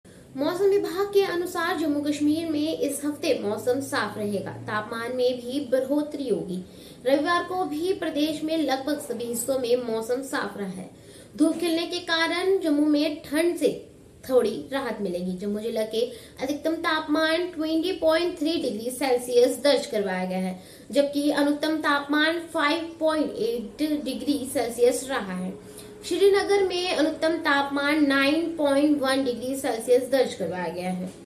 JLKMEDIA जम्मू-कश्मीर: प्रदेश में इस सप्ताह साफ रहेगा मौसम
JLKMEDIA जम्मू-कश्मीर: प्रदेश में इस सप्ताह साफ रहेगा मौसम, श्रीनगर का अधिकतम तापमान  9.1 डिग्री पहुंचा